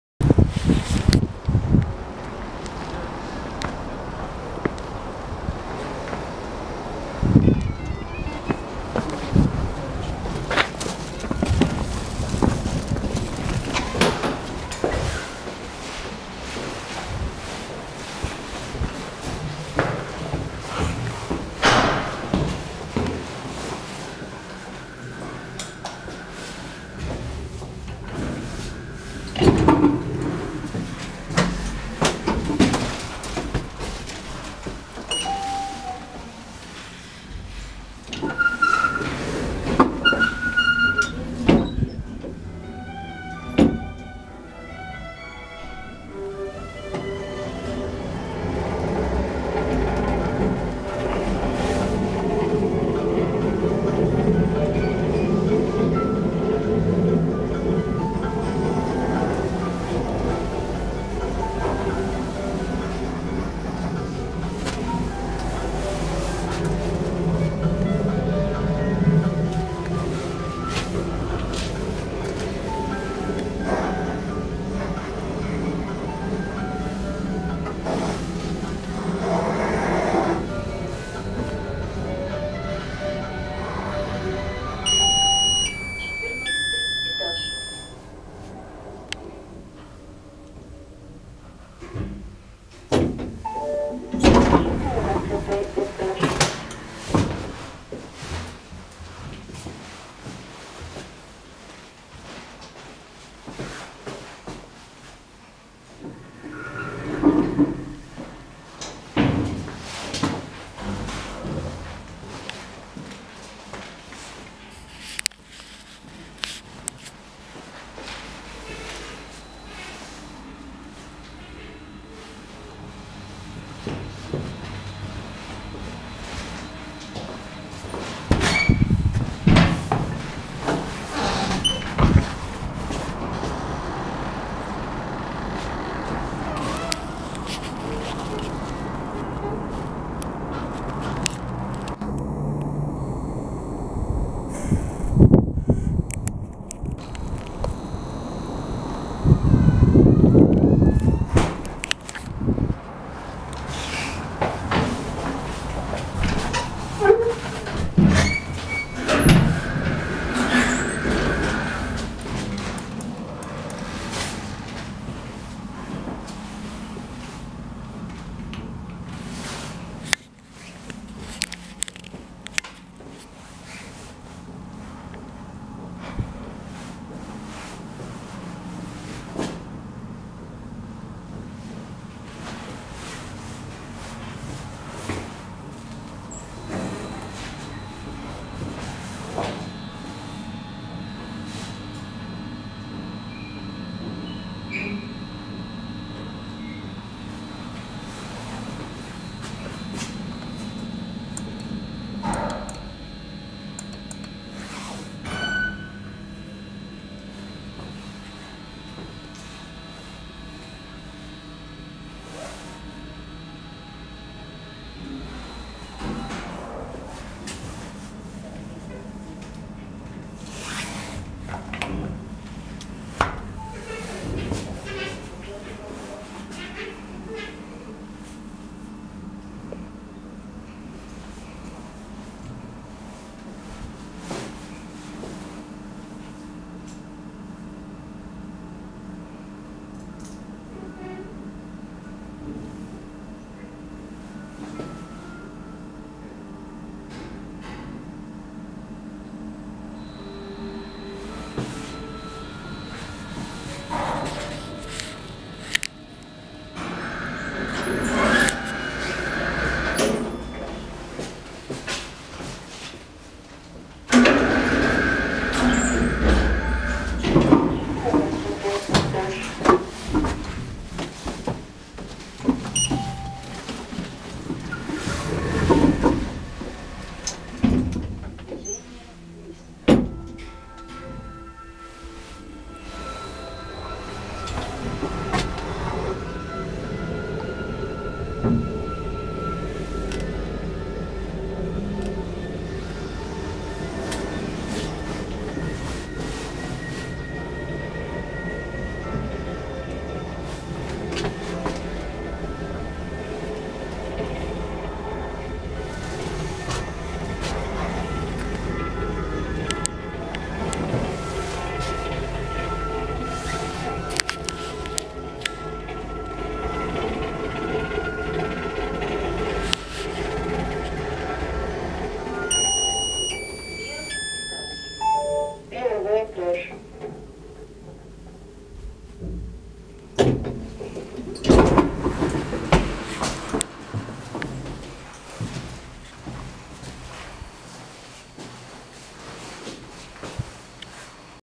Музыкальный лифт КМЗ (2019 Г.В) Г/П 630 КГ В Башне Вулыха
Адрес : Казань, улица Декабристов, 113 (Московский район)
Год постройки: 1972
Количество этажей: 14
Жилых помещений: 104
Серия дома : Башня Вулыха.
Оператор : Марк Вассаев
Модель камеры : Canon PowerShot 540 HS
Группа Карты Ульяновска в Omsi 2 :
Плейлисты с лифтами :